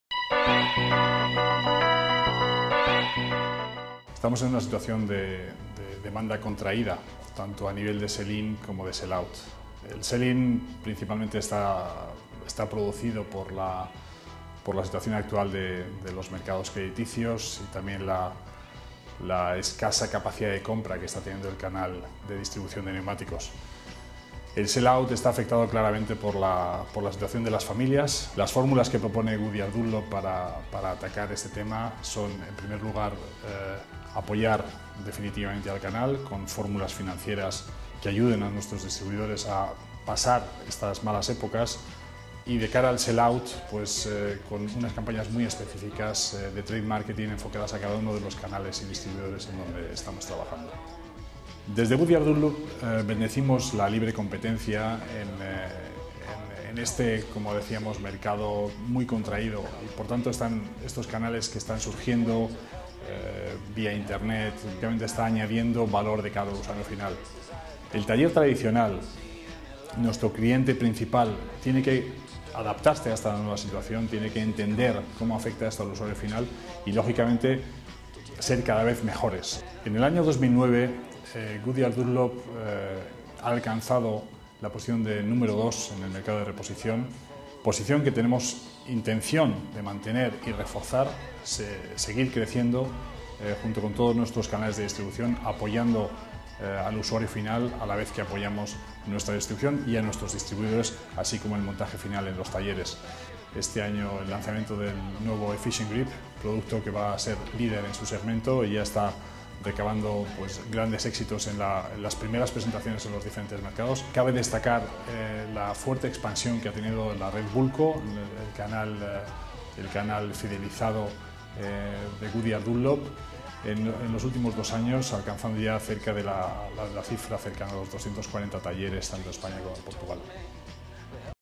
Darío Vicario (Goodyear Dunlop)
InfoCap TV entrevista a Darío Vicario, director general de Goodyear Dunlop Iberia. Visita el portal líder para talleres de reparación de automóviles.